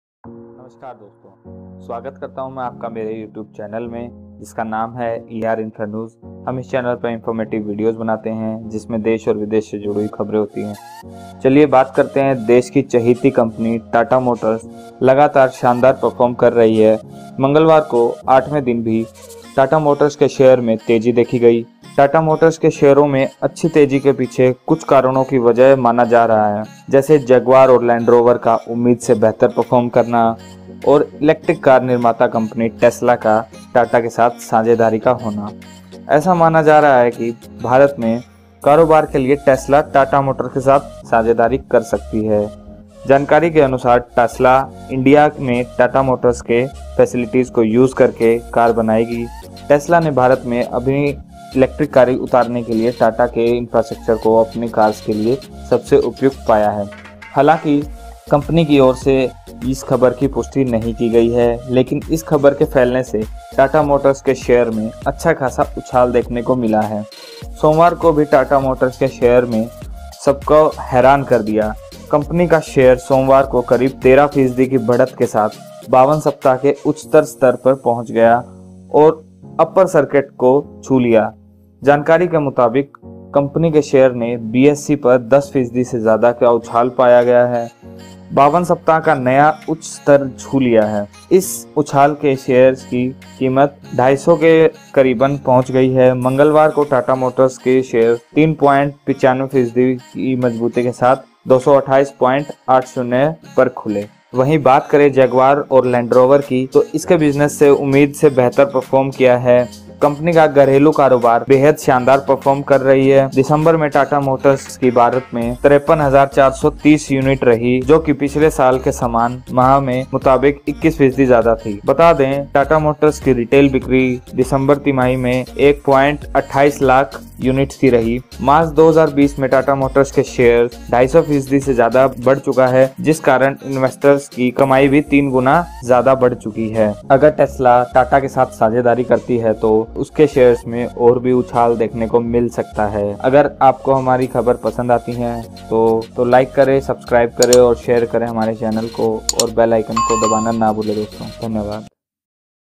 Tata's collaboration with Tesla?
Tata Motors के शेयर ने छुआ 52 हफ्ते का हाई, इन वजहों से आई तेजी ||